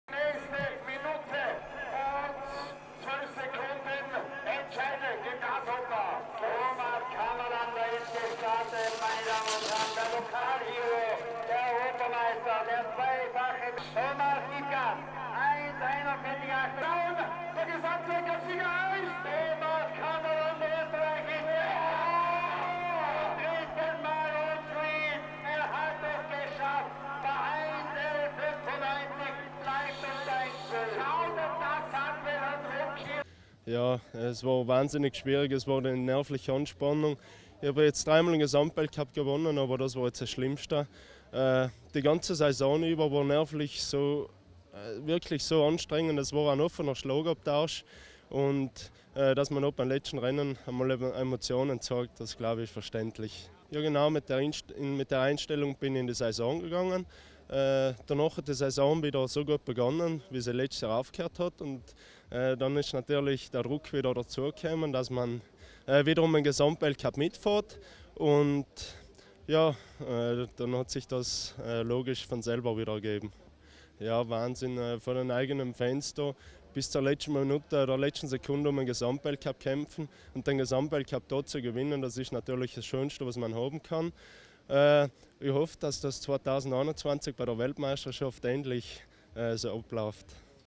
Thomas Kammelander | dritter Gesamtweltcupsieg in Folge | Umhausen 2019 | Rodel Austria
Thomas Kammerlander sicherte sich beim Weltcupfinale der Naturbahnrodler mit seinem zweiten Platz den dritten Sieg im Gesamtweltcup in Folge. Der Sieger im Ötztal hieß dieses mal allerdings Patrick Pigneter, Platz 3 ging  an Alex Gruber.